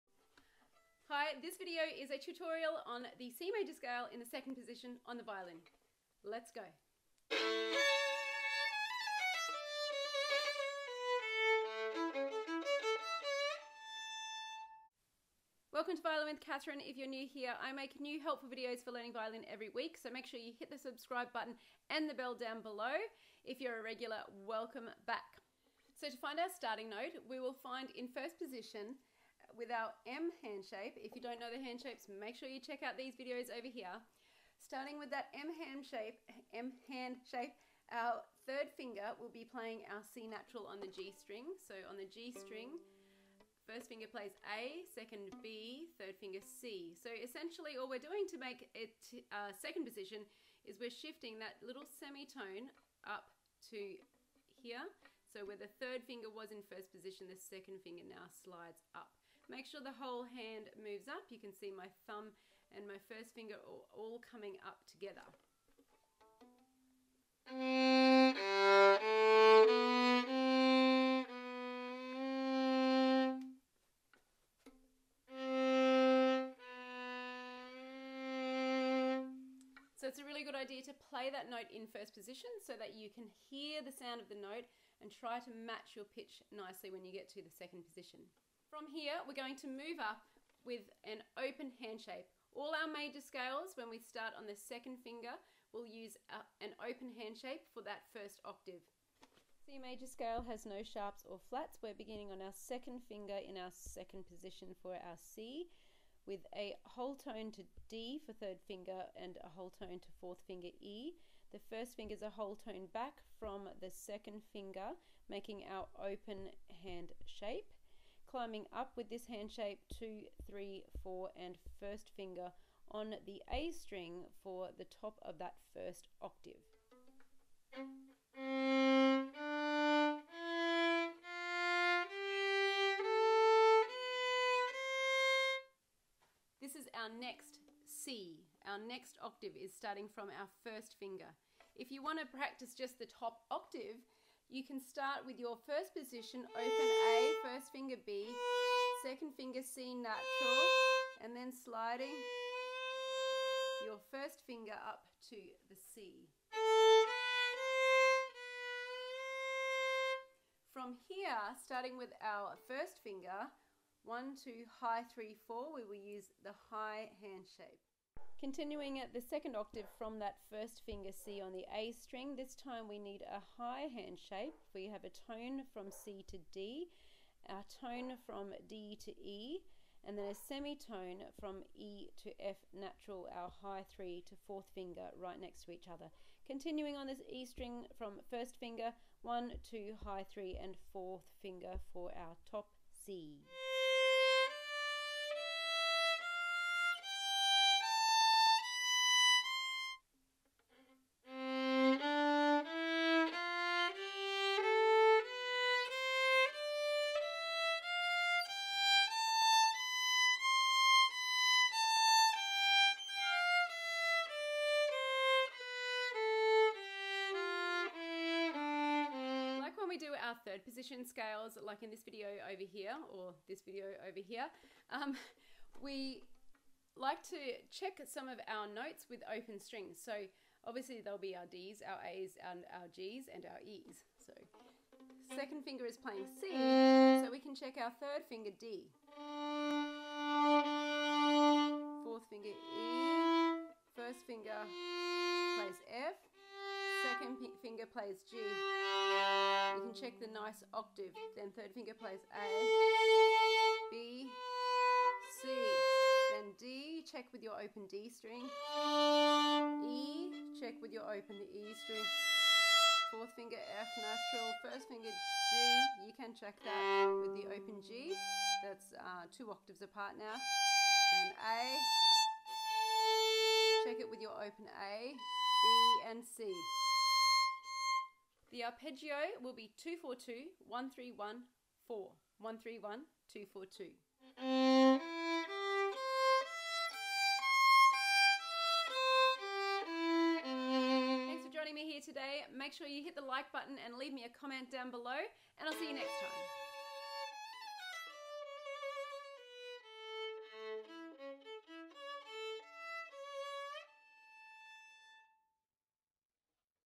Violin C Major Scale, Violin 2nd Position, Violin 2 Octave Scales
This video is a tutorial on the C major scale in 2nd position. This is a 2 octave scale, and as always, I will also show you how to play the matching arpeggio.
I’ll also give you some pointers to practice for improving your pitch through this scale, so be sure to watch the whole video so you don’t miss any helpful tricks.